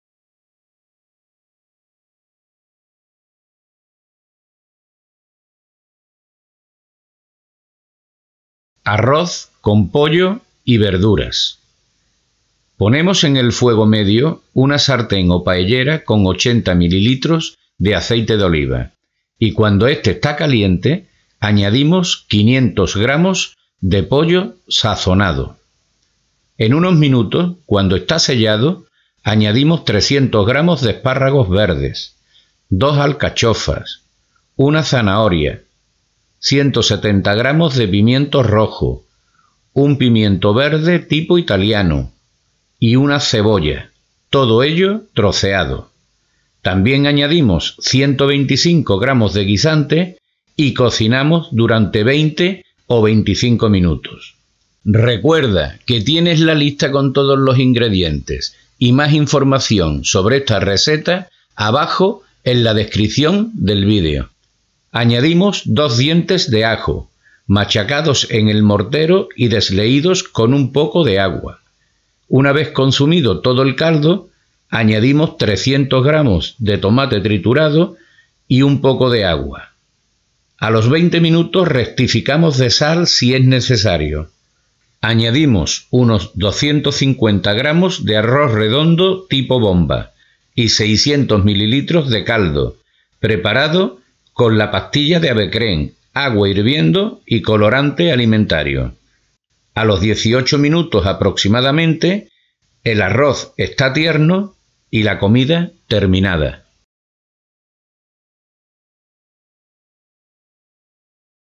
Arroz con pollo y verduras - Cocina andaluza y española | Andalucía Videorecetas
Arroz con pollo y verduras una receta de cocina tradicional andaluza y española.

Esta es una segunda edición corta con sonido mejorado de nuestra vídeo receta de cocina original con el listado de ingredientes y cantidades